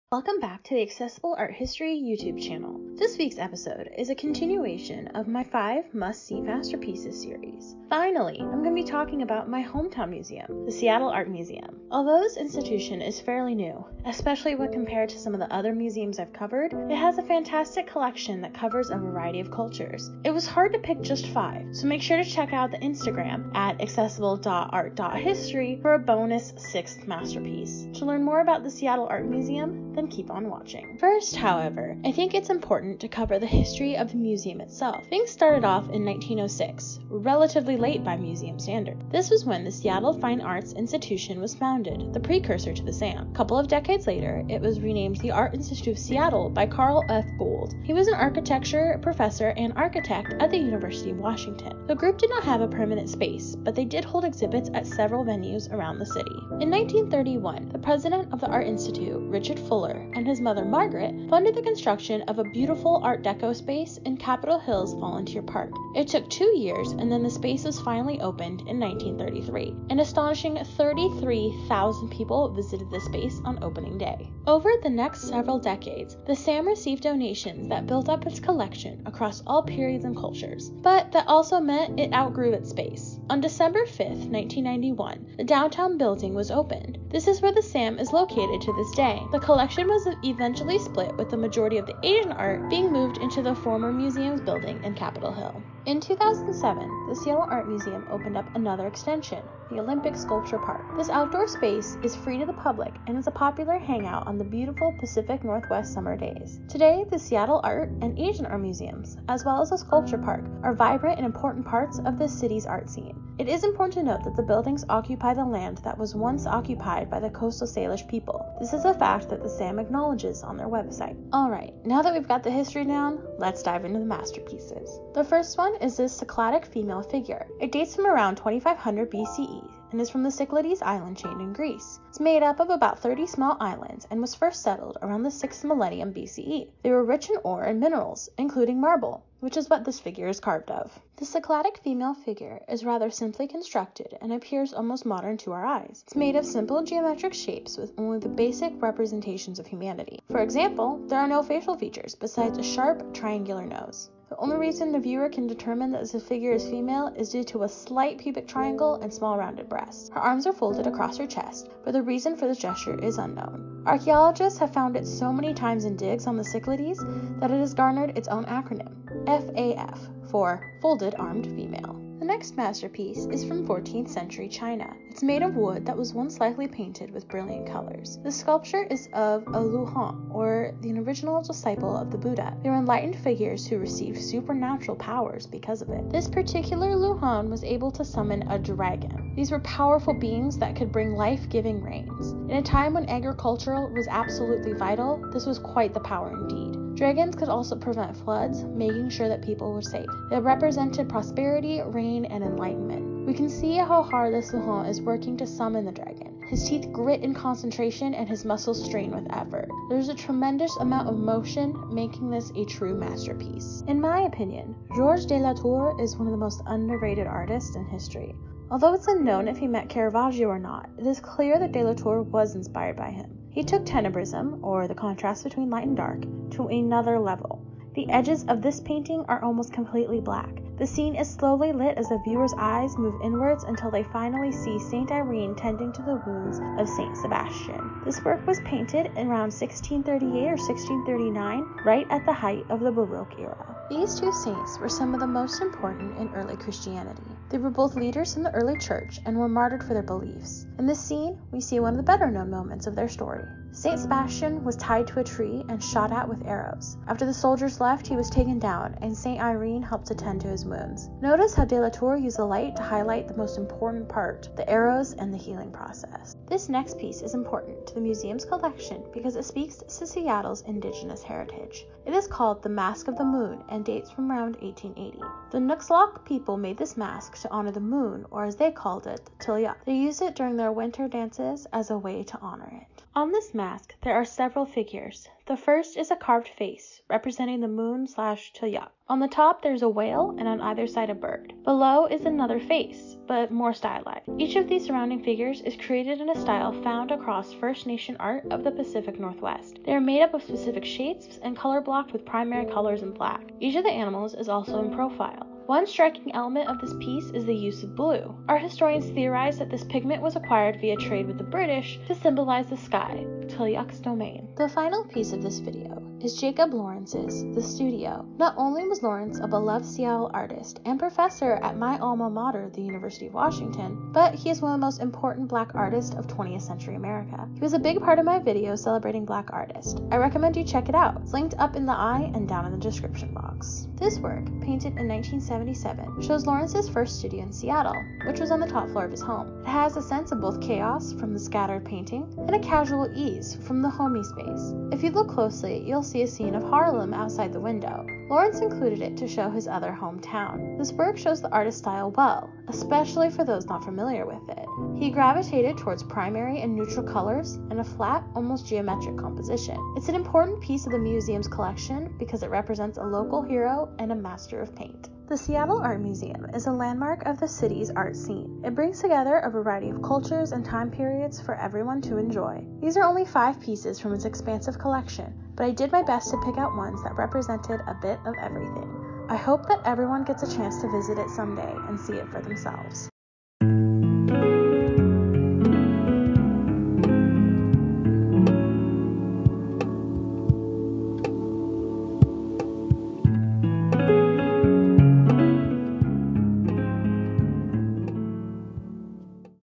Five Must See Masterpieces at the Seattle Art Museum
The Seattle Art Museum has an extensive collection that covers multiple cultures and time periods. Come learn all about it in this video by Accessible Art History!

Sources